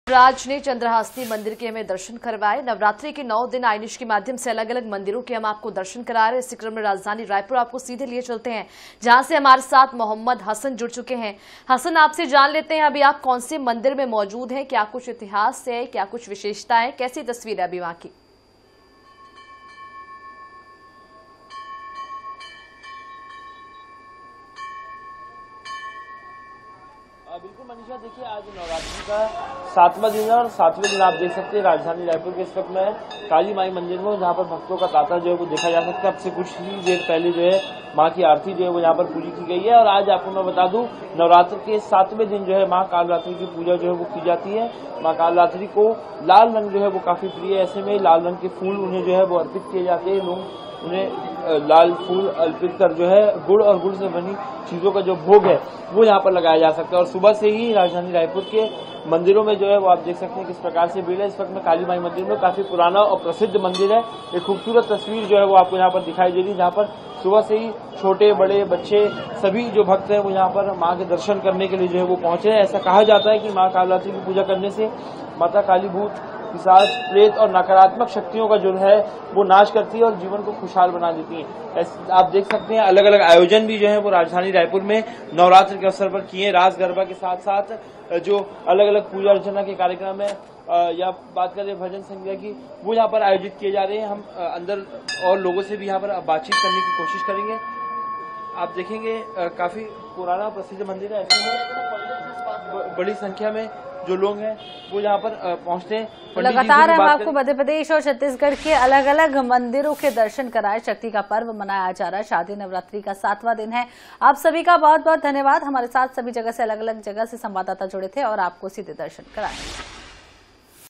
Shardiya Navratri 2024 7th Day: मां कालरात्रि का दिन, मंदिरों में भक्तों का सैलाब
INH, Shardiya Navratri 2024 7th Day: मां कालरात्रि का दिन, मंदिरों में भक्तों का सैलाब